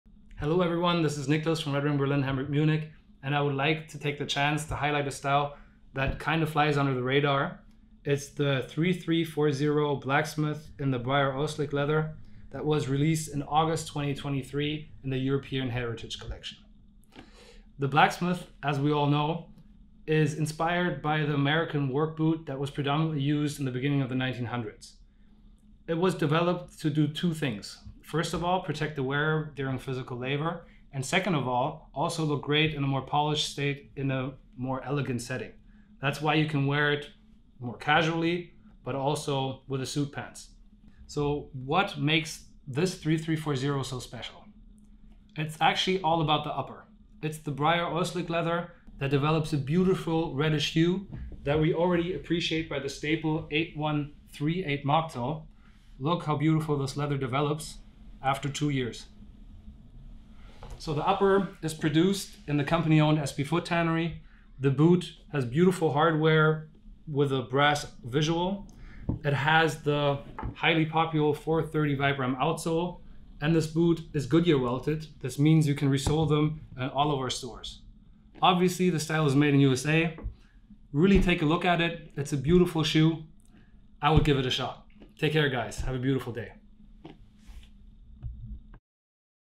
What makes the new Red Wing Blacksmith 3340 special?
What makes the new 3340 Blacksmith in Briar Oil Slick so special? Nicklas dives into the details and gives you a little insight into the features of this recent release!